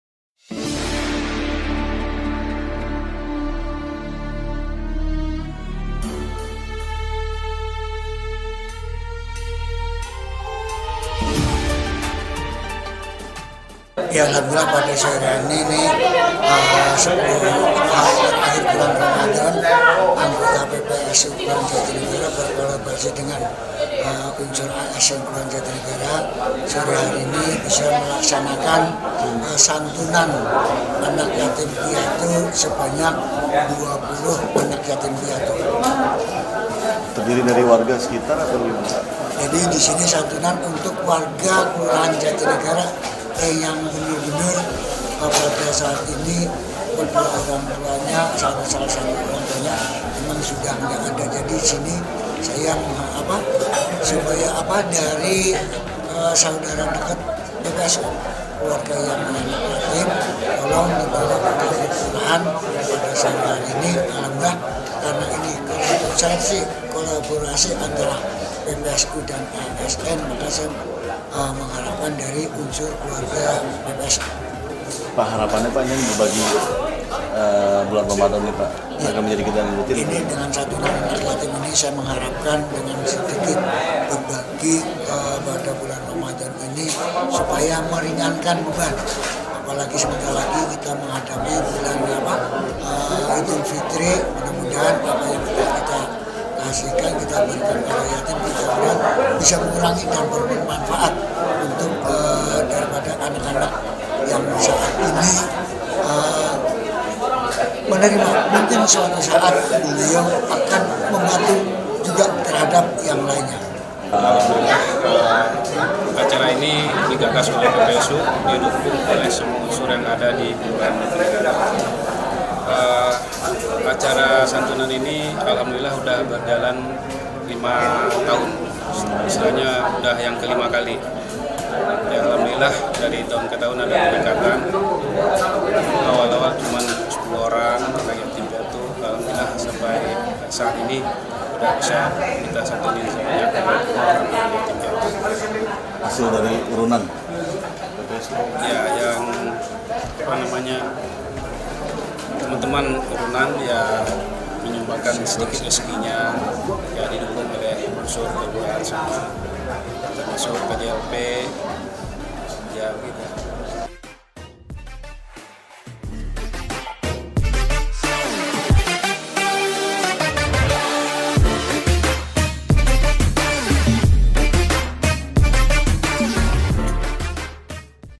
Berbagi Kebahagian, PPSU Jatinegara Santuni Puluhan Yatim Piatu
Petugas Penanganan Prasarana dan Sarana Umum (PPSU) Kelurahan Jatinegara, Kecamatan Cakung, Jakarta Timur memberikan santunan bagi 20 anak yatim piatu di lingkungan warga Kelurahan Jatinegara.

Santunan tersebut diserahkan Lurah Jatinegara Slamet Sihabudin di Aula Serbaguna Kantor Kelurahan Jatinegara